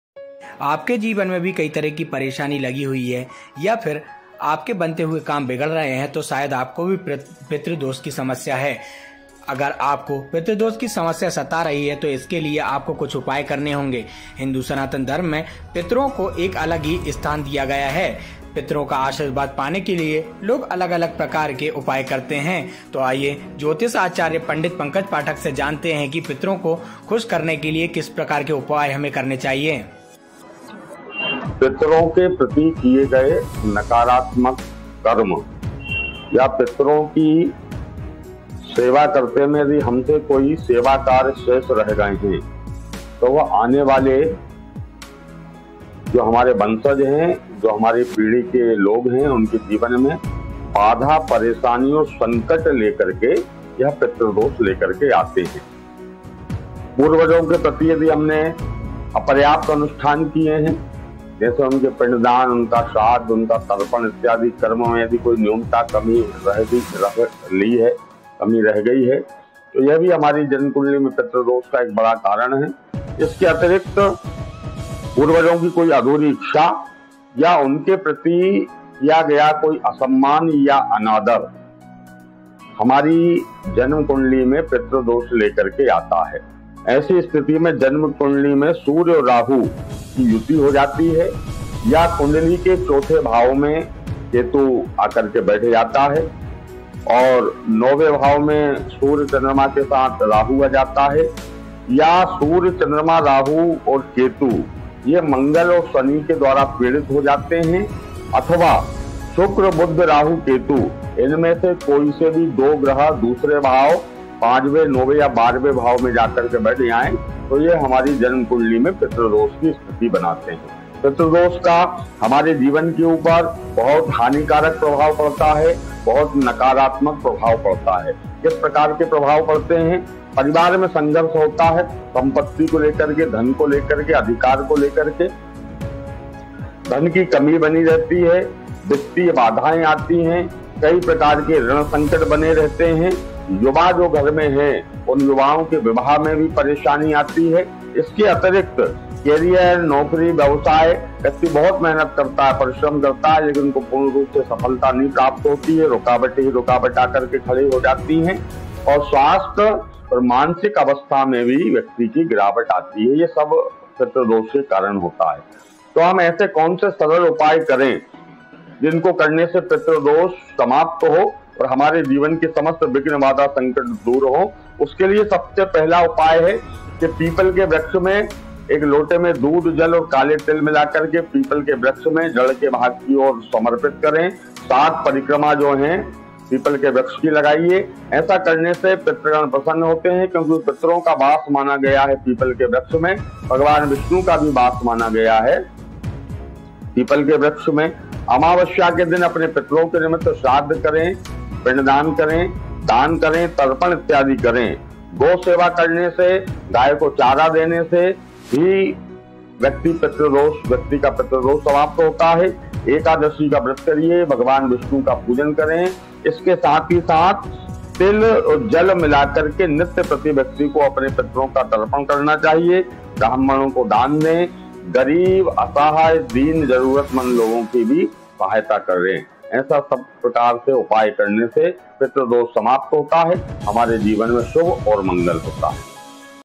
Pitra Dosh Upay: पितृ नाराजगी के संकेत, पहचानें और जल्द करें ये उपाय!
If you are facing many kinds of problems in your life, or your work is getting spoiled, then perhaps you have the problem of Pitru Dosh. If you are facing the problem of Pitrudosh. So for this you will have to take some measures. In Hindu Sanatan Dharma, ancestors have been given a different place
आपके जीवन में भी कई तरह की परेशानी लगी हुई है, या फिर आपके बनते हुए काम बिगड़ रहे हैं, तो शायद आपको पितृदोष की समस्या है. अगर आपको पितृदोष की समस्या सता रही है. तो इसके लिए आपको कुछ उपाय करने होंगे. हिंदू सनातन धर्म में पितरों को एक अलग ही स्थान दिया गया है.